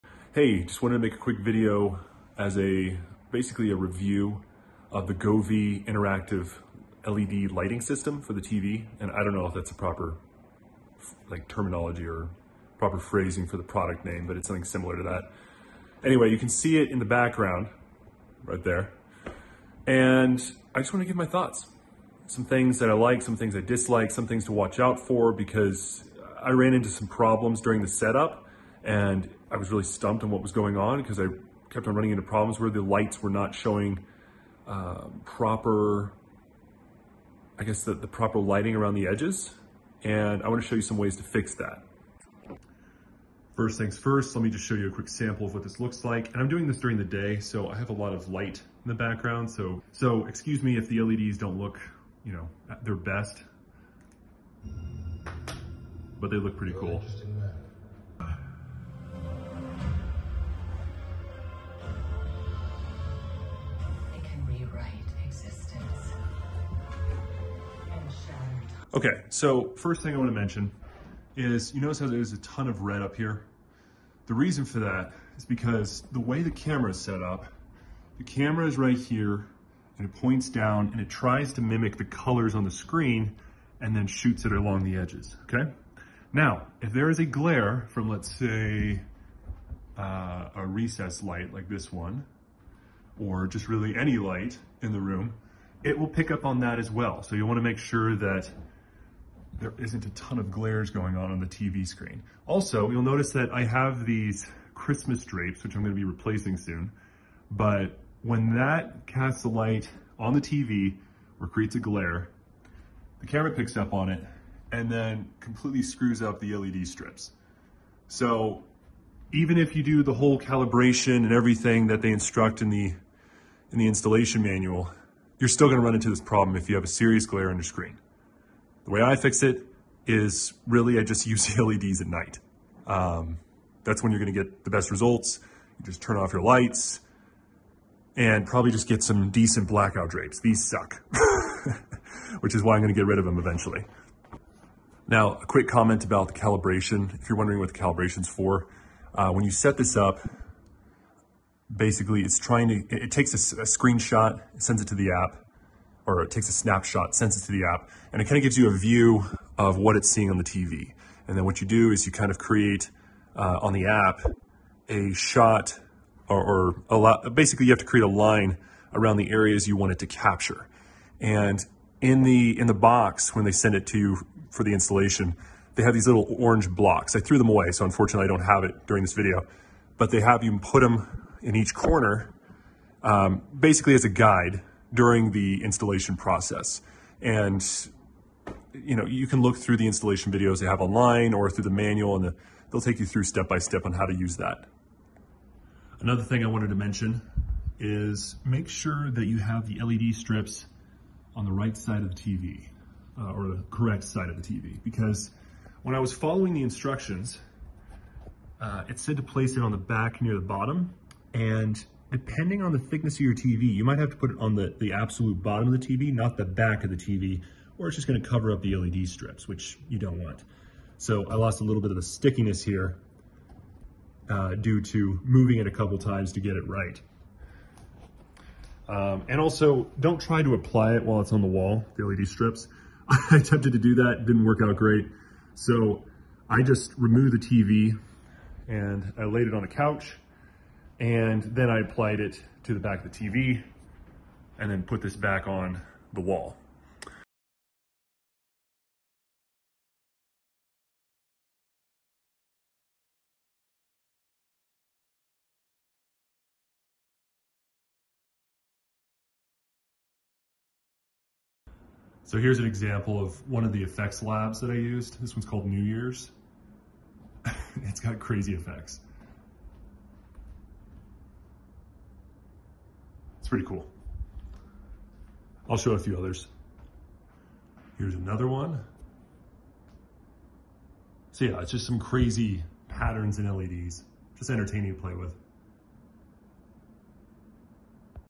Govee Envisual TV LED Backlights with Camera, DreamView T1 REVIEW
In this video I'll be reviewing the Govee Envisual TV LED Backlights with Camera, DreamView T1.